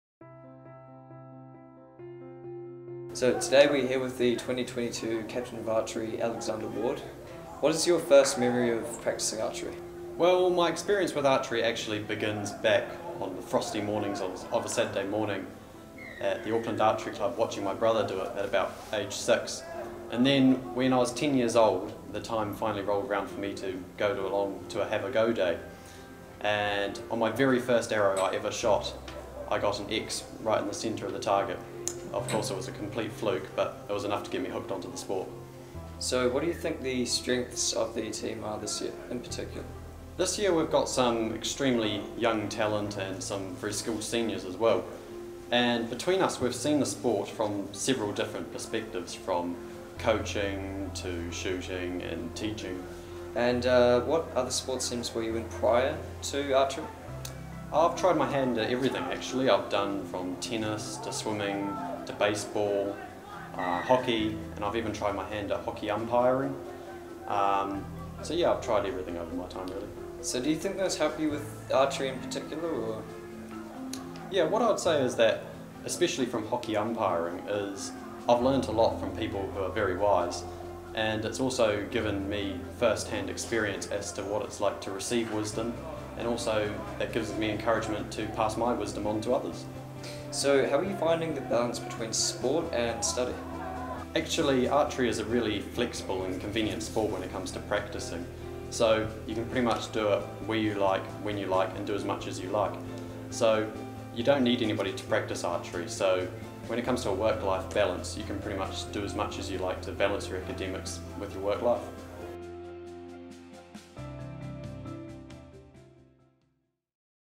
Sports Captain interview - 2022 Captain of Archery
Throughout Term 1, members of the Headmaster's Heritage Committee have interviewed our summer sports captains. After getting a bullseye from his very first arrow, 2022 Captain of Archery Alexander Ward was hooked. He spoke to Committee member Hamish Covacich about his start with the sport at age 6, the strengths of this year's team and how the other sports he has tried while at Auckland Grammar School have helped him hone in on his skills at Archery